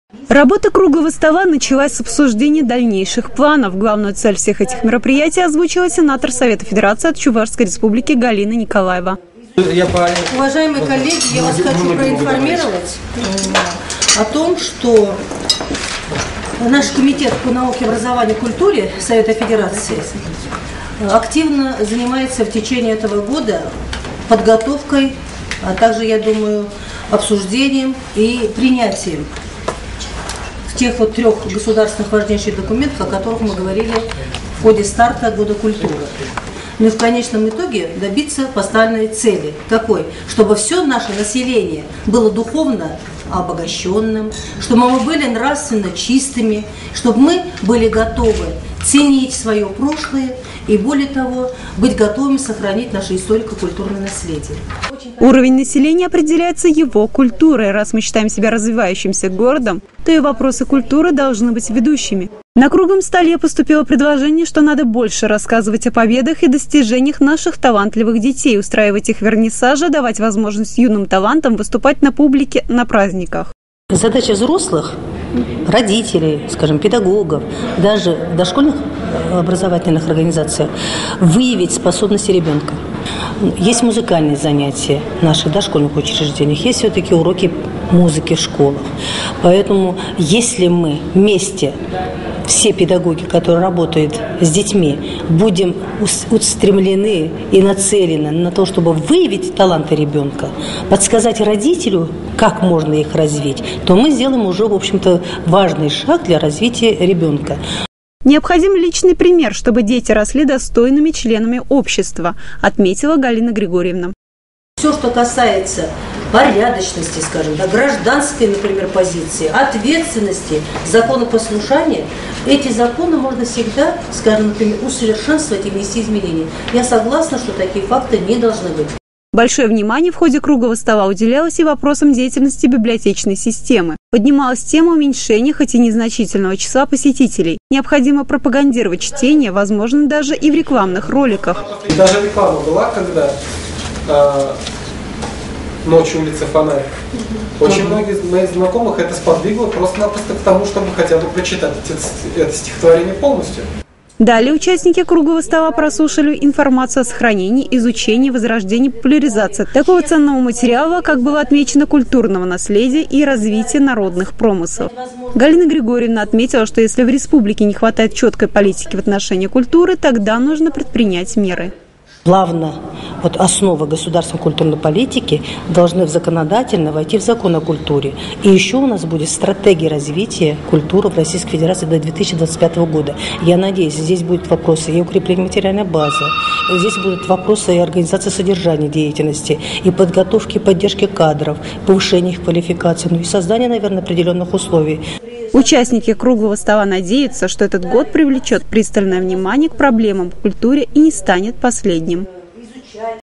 В Новочебоксарске прошёл круглый стол по вопросам культуры
Собрать и выслушать каждого. 9 июня за круглым столом в стенах Историко-музейного комплекса Новочебоксарска по вопросам культуры встретились сенатор Совета Федераций от Чувашской Республики Галина Григорьевна Николаева и известные деятели культуры и искусства Новочебоксарска. Галина Григорьевна также ознакомилась с действующей экспозицией, рассказывающей о древнейших обитателях края по данным палеонтологии.